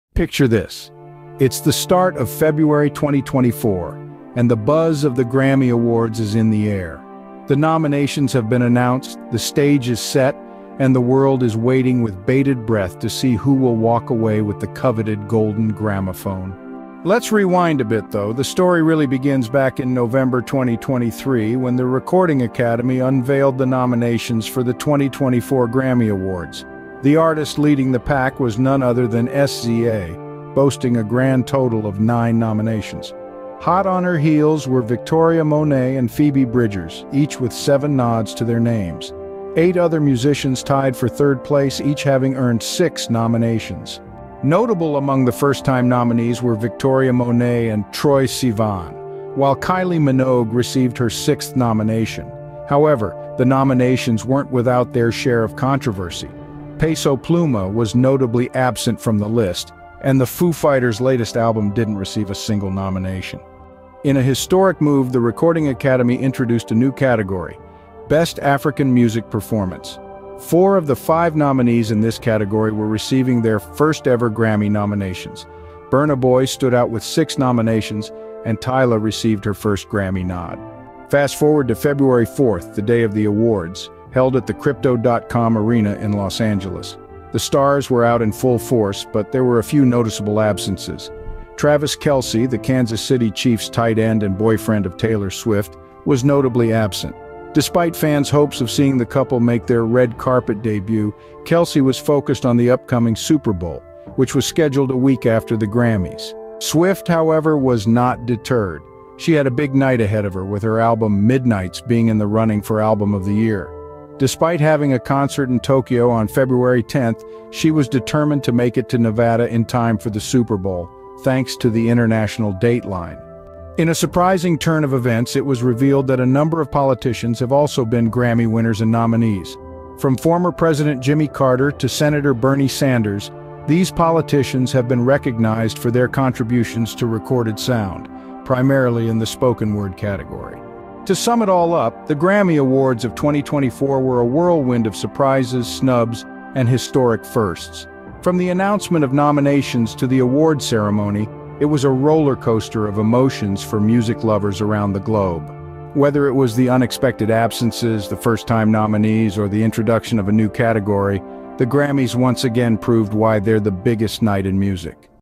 Grammy awards 2024. Highlights
72 milleti kıran bozuk düzene karşı başkaldırı, mazlumların sessiz çığlığına umut, Kuruluş’un adı oldu…

Video Description:
In this exciting video, we dive deep into the most unforgettable moments of the Grammy Awards 2024! Join us as we uncover the major events, unexpected surprises, and shocking snubs that took the music world by storm. Based on trusted news reports, we bring you an inside look at the glitz, glamour, and controversy of this highly anticipated music industry event. Whether you're a die-hard fan or just curious, this video is a must-watch for all music enthusiasts. Sit back, relax, and enjoy the star-studded journey into the Grammy Awards 2024.